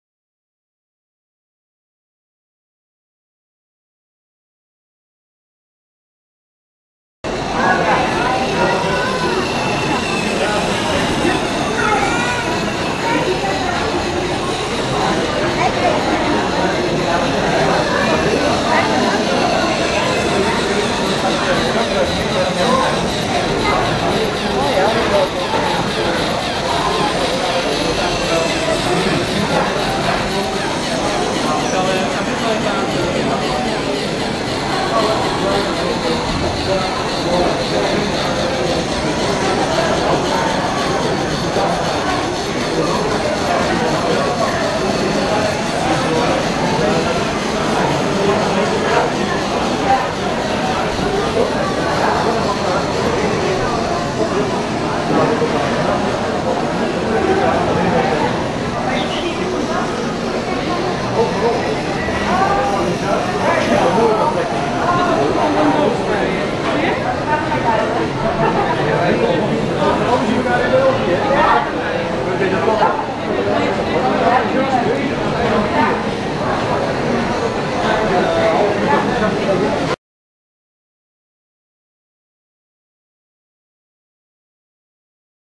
Robocup Jr 2011 at the Nemo
Mindstorms Demo at the Robocup Junior championships at the science museum, Nemo in Amsterdam, the Netherlands 28 May 2011